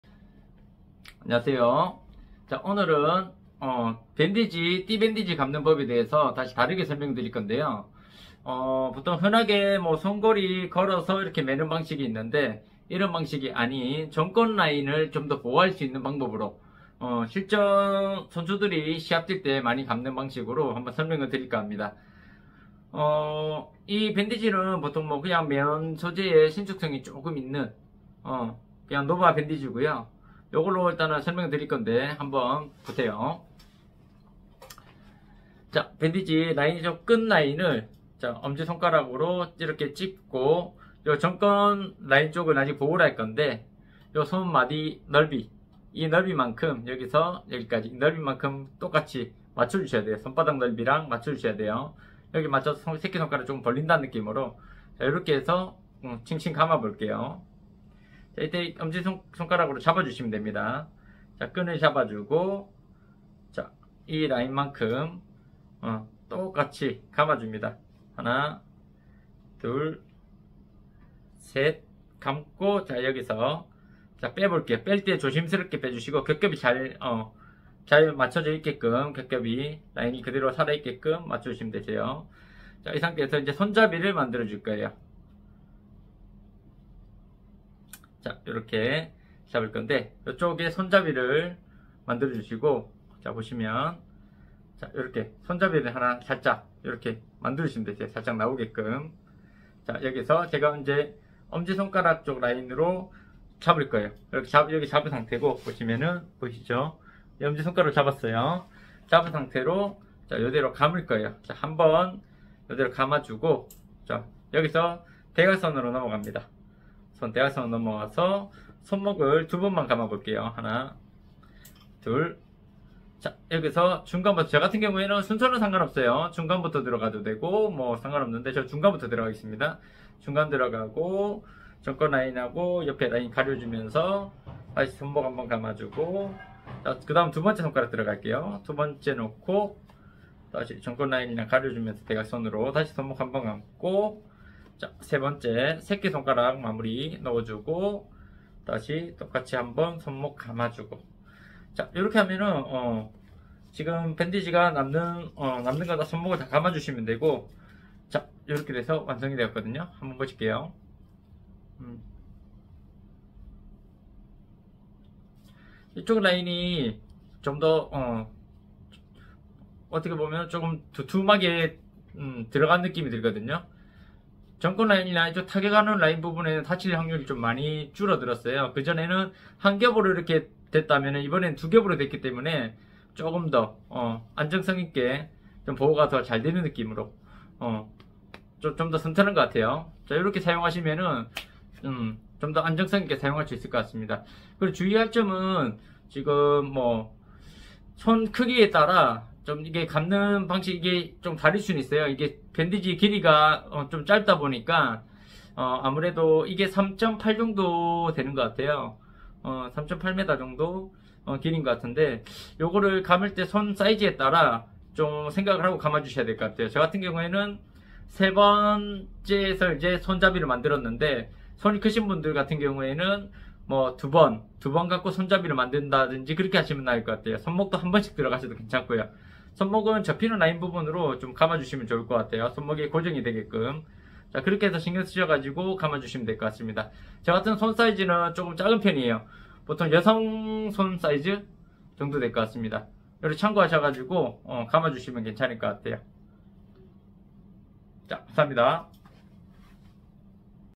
평범한 핸드랩으로?? 부상방지!! 핸드랩 감는방법!! 제대로 감아보자!!
핸드랩 감는방법은 다양합니다. 이번에는 이렇게 감아보세요~!